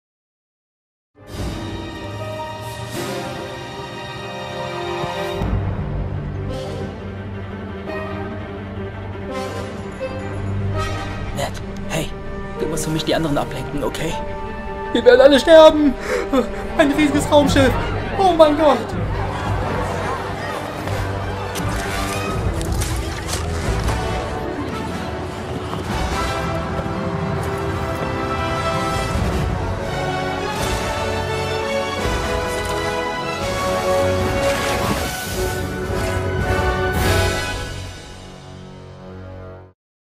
Ablenkungsmanöver (German Fandub)
Ein Kurz-Spaßfandub aus dem mcu :)
This is a non profit fanmade video for private entertainment.
marvelstudios.
Ich hoffe es gefällt euch und wünsche euch gute Unterhaltung :)